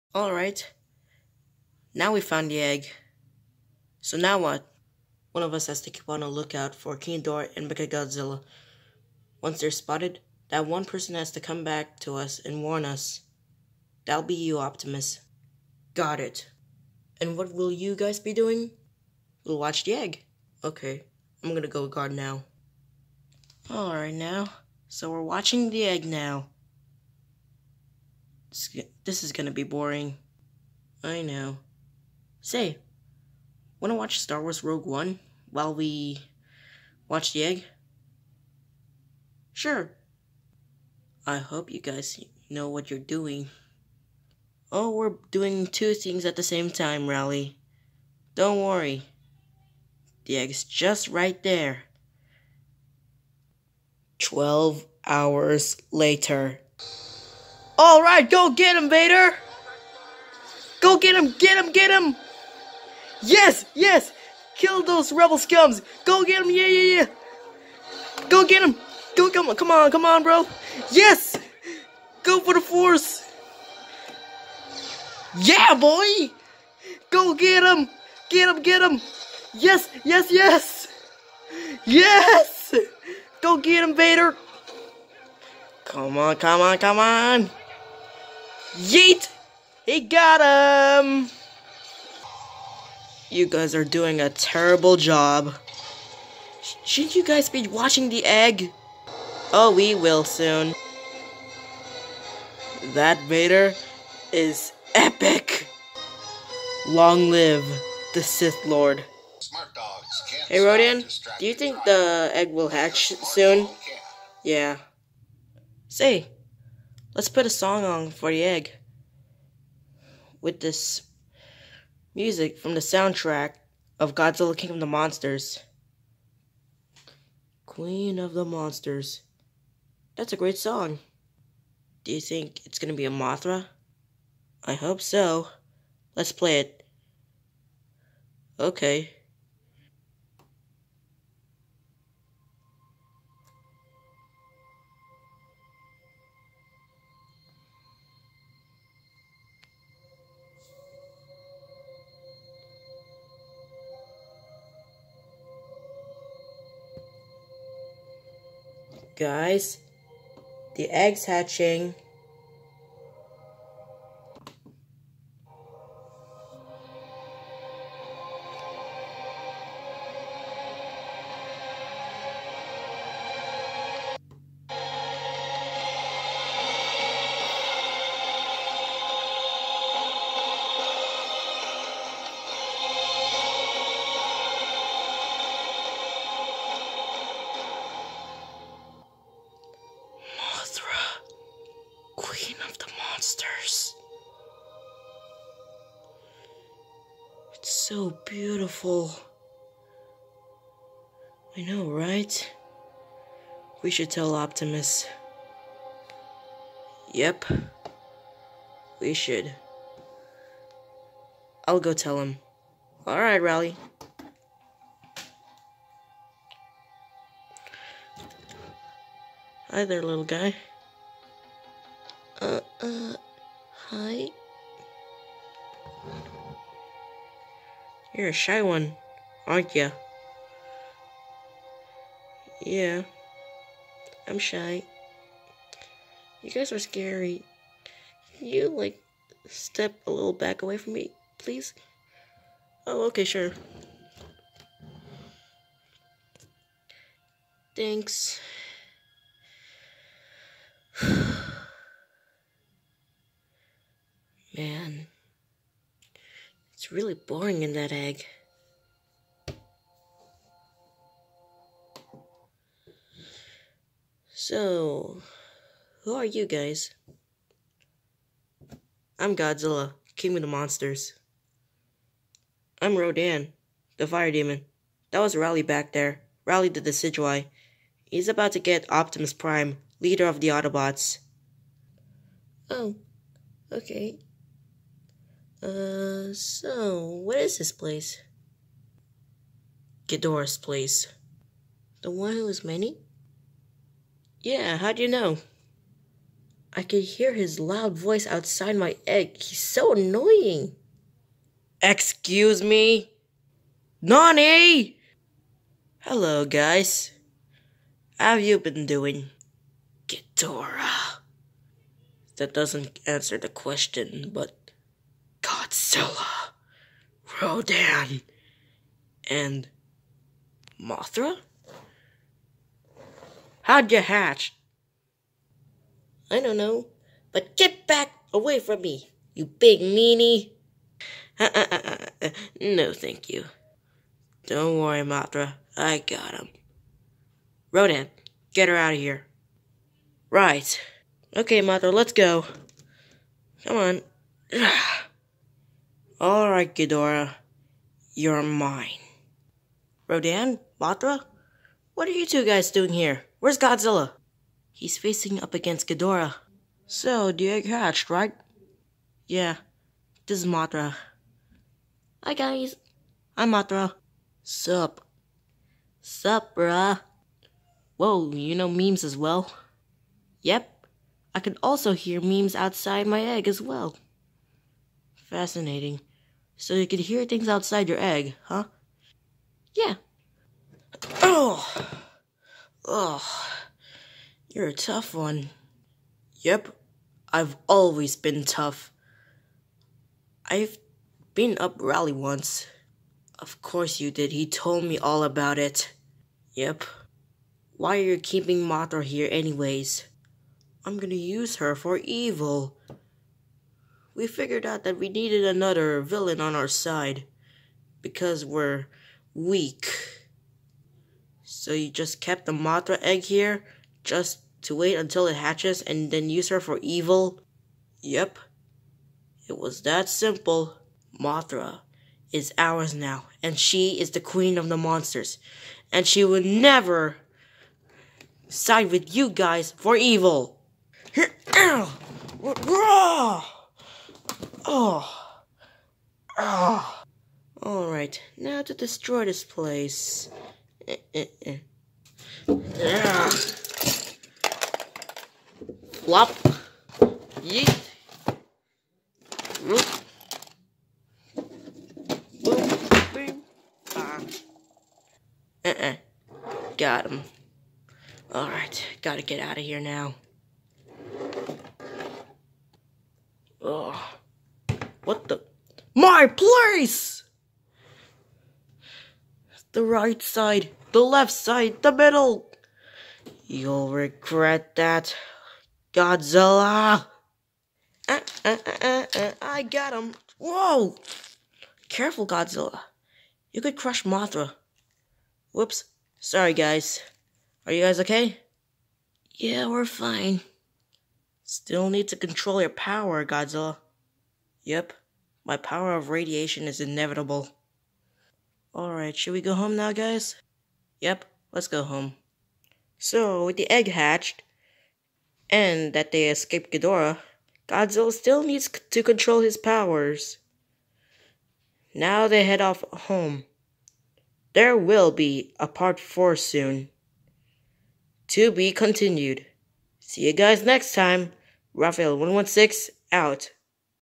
The Adventures of G.T.P - Episode 6: Godzilla's new Dorsal Fins part 3/The Birth of Mothra.
After finding the Egg, Godzilla, Rodan, and Rowly watch the egg while Optimus Prime keeps a look out for any villains, and Godzilla's still learning how to control his power of Radiation, Part 4 will be coming up soon.